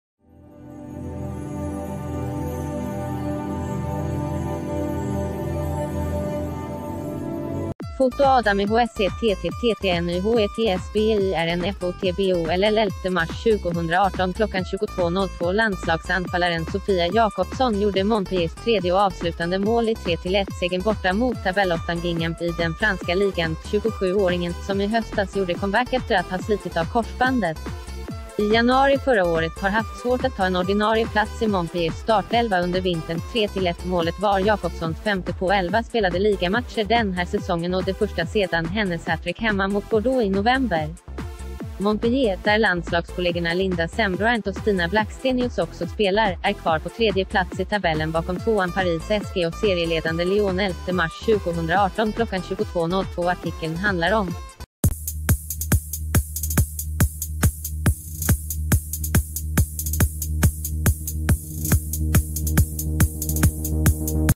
Breaking News | Sofia Jakobsson bröt måltorka i Montpellier-seger
Foto: ADAM IHSE / TT / TT NYHETSBYR≈NFOTBOLL11 mars 2018 22:02Landslagsanfallaren Sofia Jakobsson gjorde Montpelliers tredje och avslutande mål i 3–1-segern borta mot tabellåttan Guingamp i den franska ligan.27-åringen, som i höstas gjorde comeback efter att ha slitit av korsbandet i januari förra året, har haft svårt att ta en ordinarie plats i Montpelliers startelva under vintern.3–1-målet var Jakobssons femte på elva spelade ligamatcher den här säsongen och det första sedan hennes hattrick he...